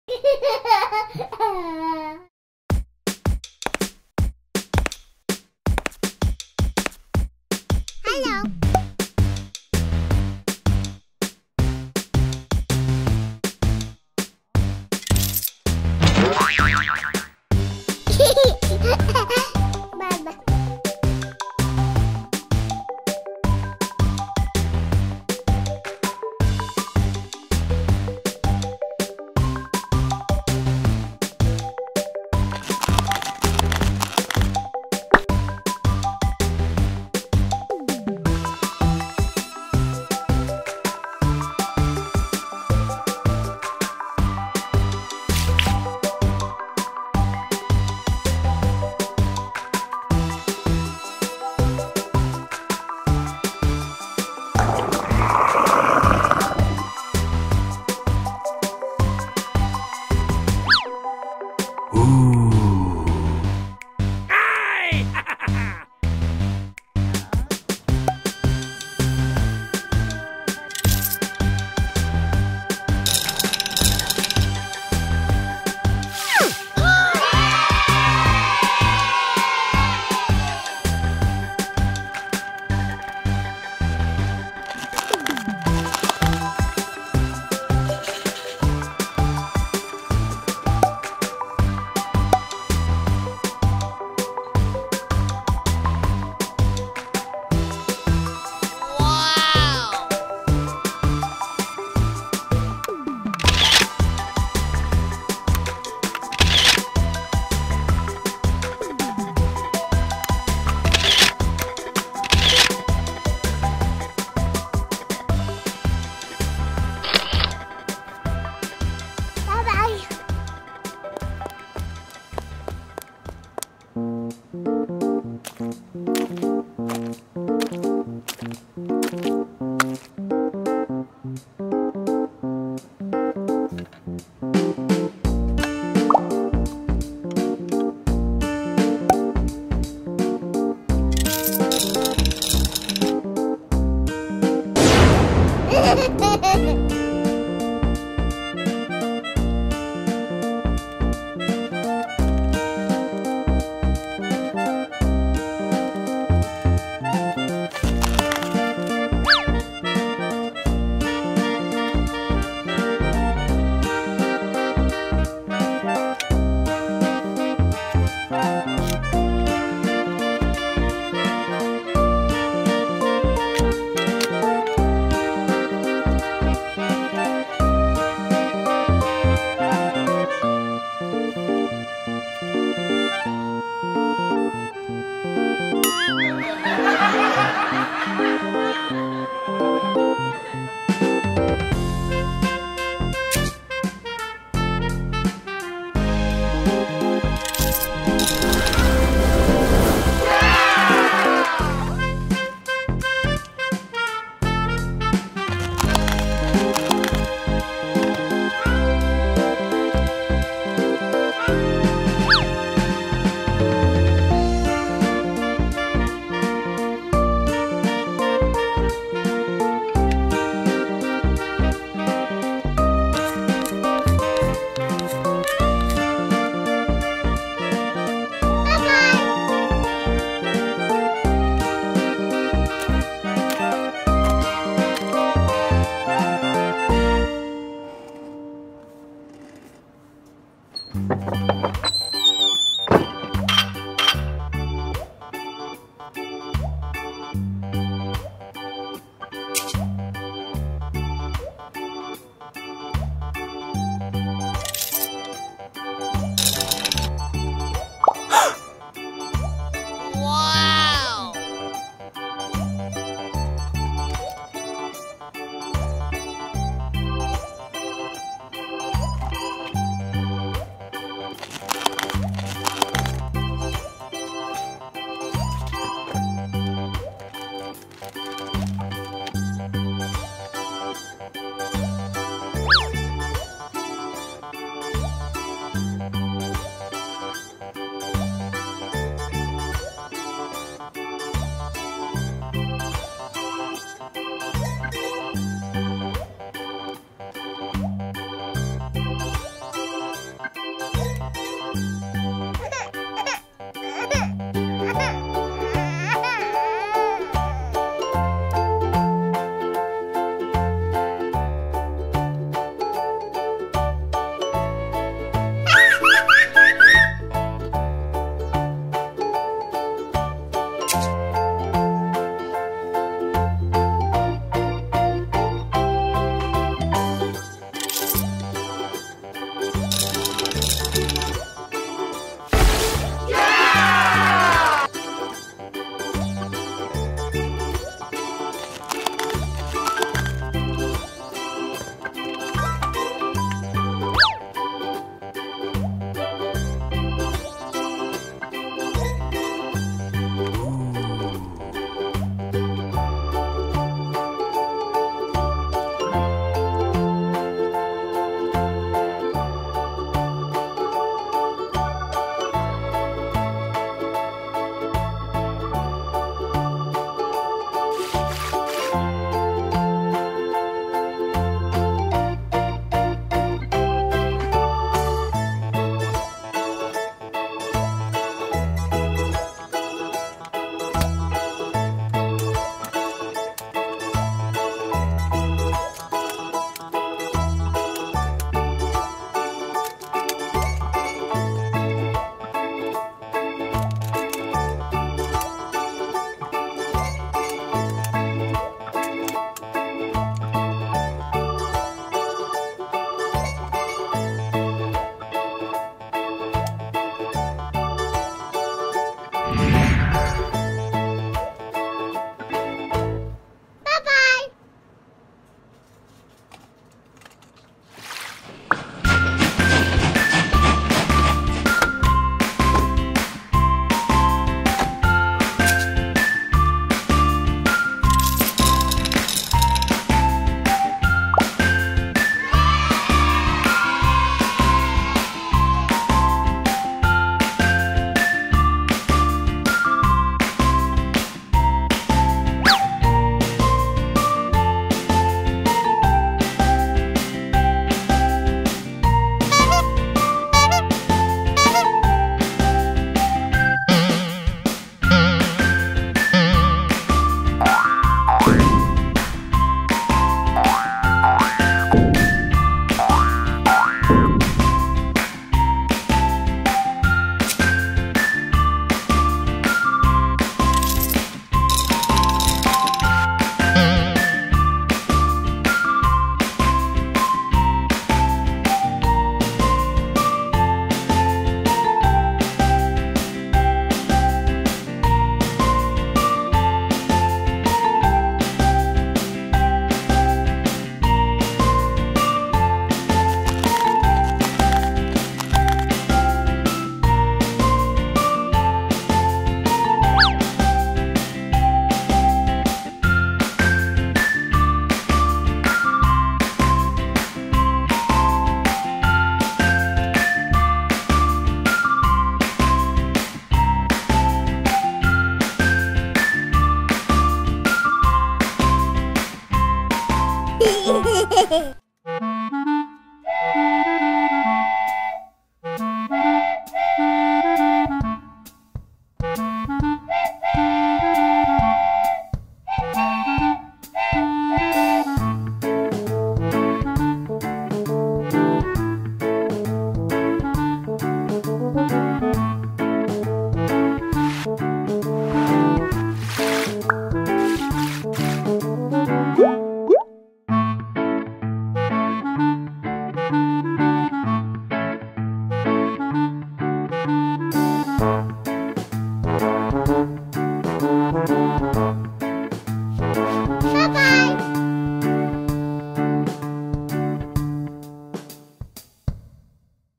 McDonald's Happy Meal Hotel Transylvania Gachapon[해피밀 몬스터호텔 가챠폰 놀이]
어른들도 아이들도 좋아라하는 가챠폰
솔라와 친구들은 어떤걸 뽑았을까요??

A Gachphon that adults and children love
What did Sola and her friends pick?
I hope you enjoy it and request your subscription.^^